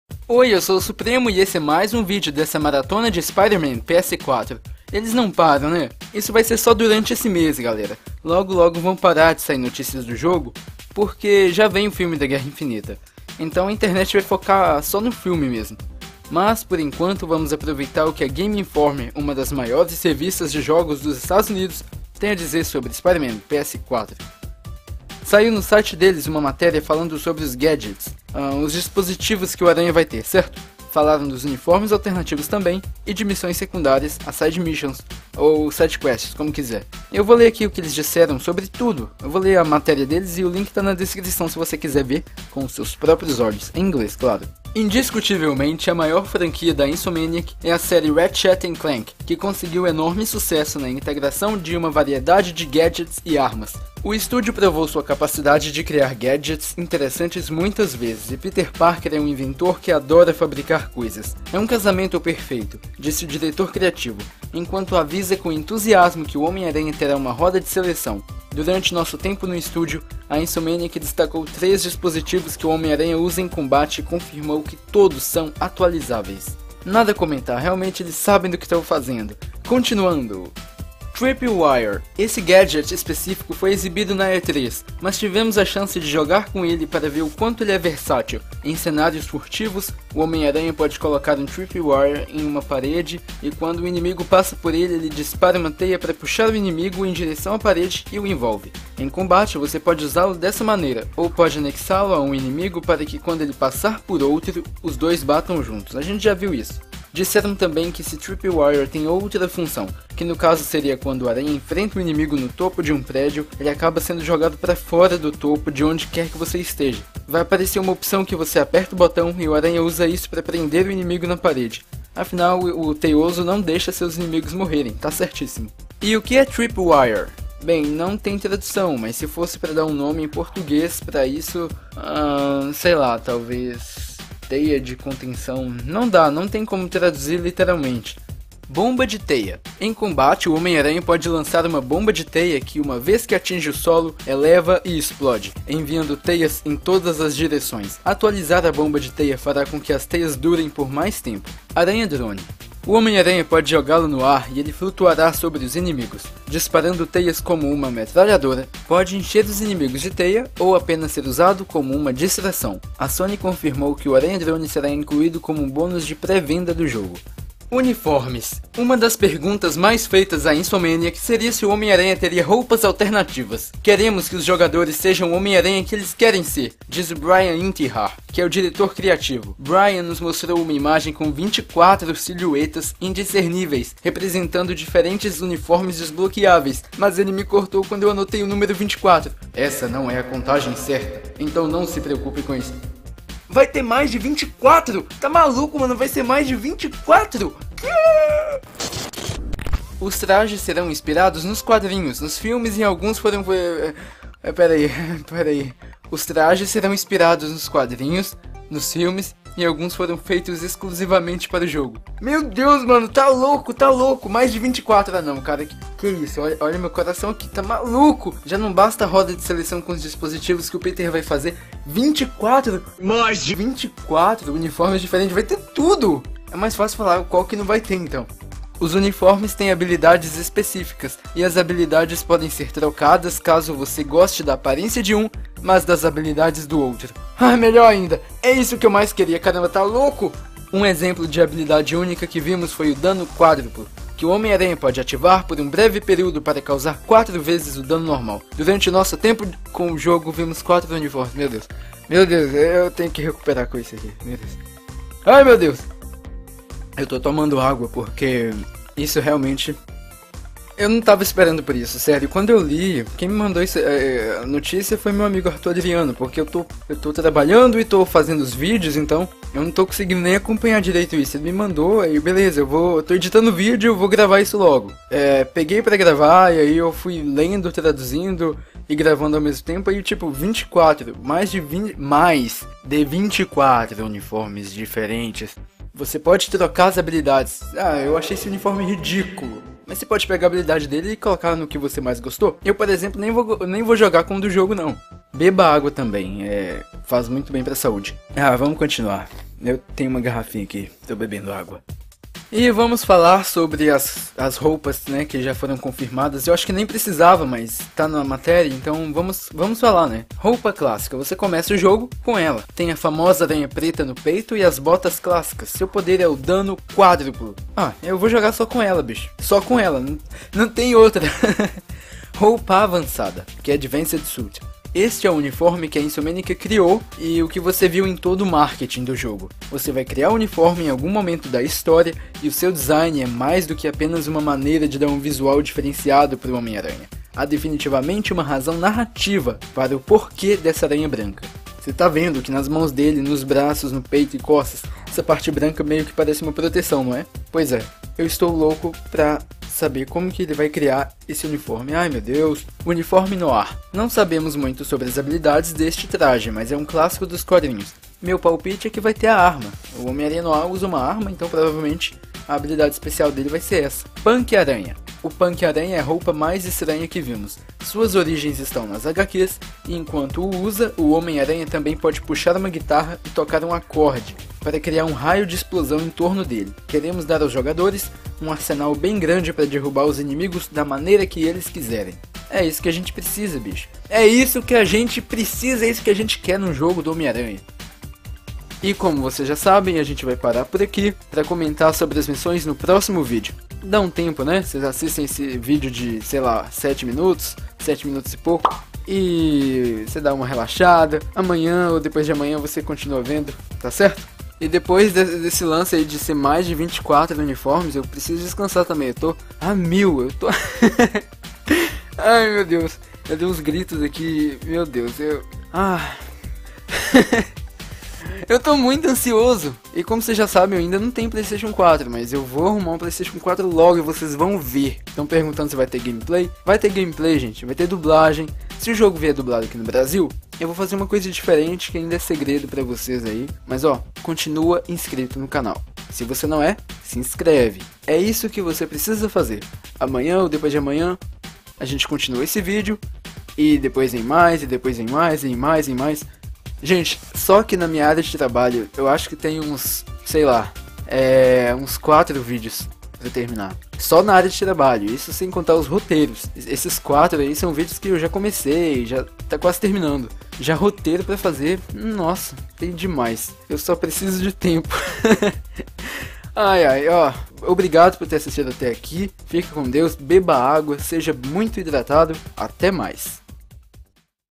Dispositivos, seleção de ataques, Spider-Drone e mais de 24 uniformes!! - SPIDER-MAN PS4 2018
CUIDADO! Esse vídeo contém alto índice de empolgação que resulta em leves gritos agudos de descrença nas notícias! Serão mais de 24 uniformes alternativos neste jogo, alguns de filmes, outros de quadrinhos e outros originais e exclusivos do game! Sem contar os gadgets, ou dispositivos, que foram confirmados. Spider-Drone? Vai ter. Serão muitos acessórios! Teia de impacto? Vai ter. Tem gente comparando com o ataque que tem nos jogos de PS1, mas pra mim é hiper parecida com a do jogo Friend or Foe, onde você joga a bola gigante de teia e ela explode, lançando teias em vários inimigos. Por isso, foi chamada de web bomb, e não impact web. Tripwire, uniformes confirmados e muito mais... é muita novidade, e ainda vamos falar depois sobre as missões secundárias. O Harry não estará em NY no jogo, e sim... bem, vou esperar o próximo vídeo pra falar disso.

Faça parte do nosso grupo no facebook!